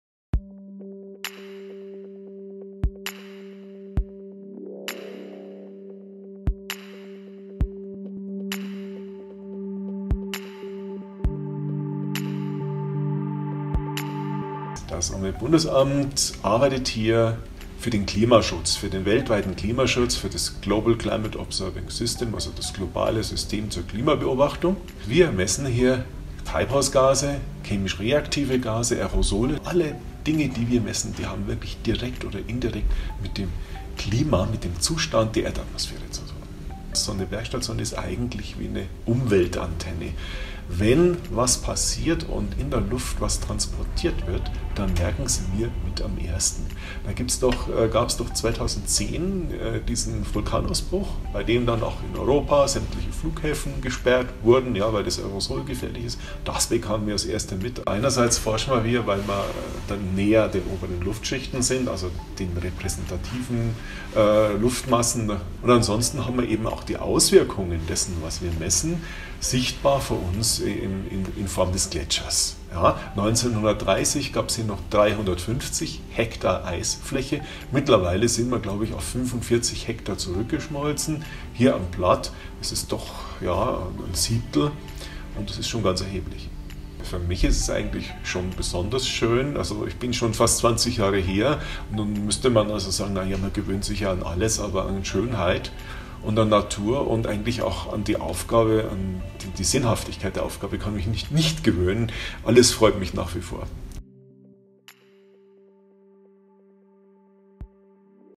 UBA-Forschung vor Ort: Schneefernerhaus auf der Zugspitze
Das Umweltbundesamt forscht im Schneefernerhaus auf der Zugspitze zum Klimawandel. Mehr zum Thema Klimawandel auf uba.de